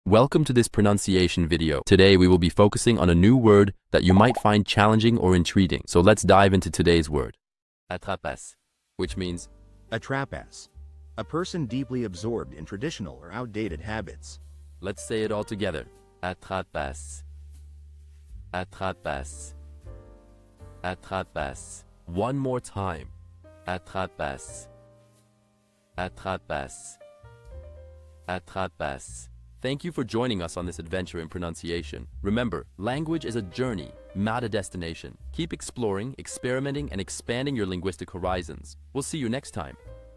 How to pronounce Attrapasse
Master the Pronunciation of 'Attrapasse Which means grabbus' - which means : Craft: A Person Deeply Absorbed in Traditional Or Outdated Habits. 📝 with @BaguettePronunciation 🎓🗣️ - [Your Guide to French]

Learn to pronounce 'Attrapasse' perfectly in French with our easy and engaging tutorial by @BaguettePronunciation! 📖📽️ In this comprehensive video, we provide you with native speaker examples 🗣️👂 and phonetic breakdowns 🎵 to ensure you get the pronunciation just right.

How to say Grab in French